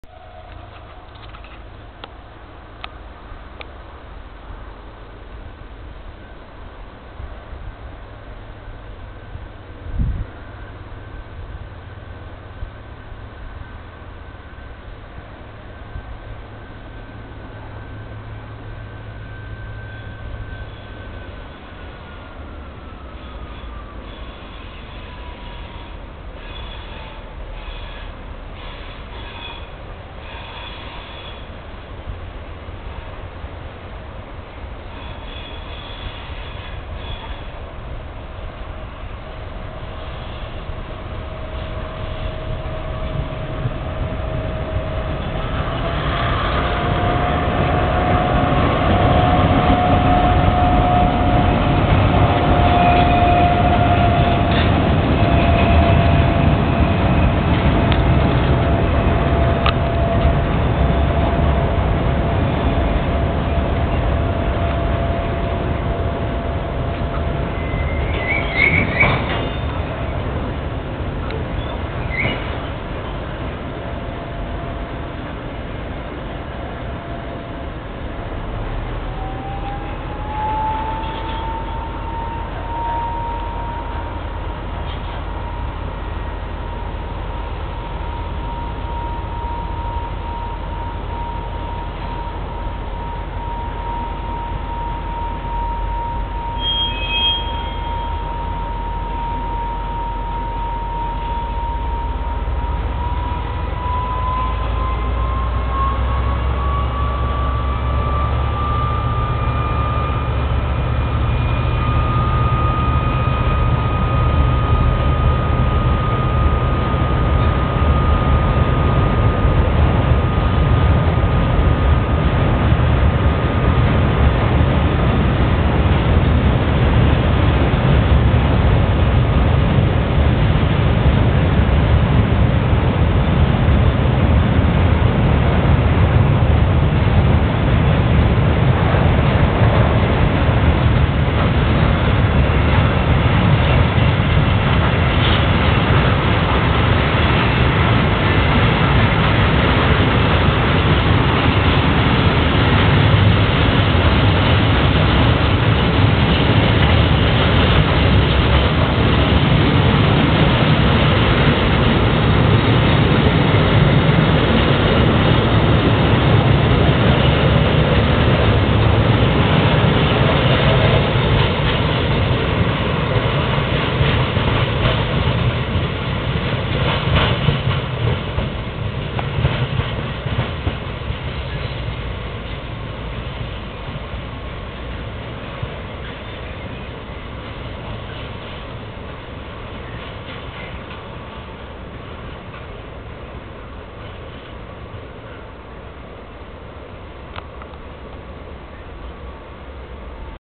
HzL-Güterzug in Hanfertal
Mo. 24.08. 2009 fährt ein Hzl-Güterzug durch den Bahnhof Sigmaringen-Hanfertal Richtung Bingen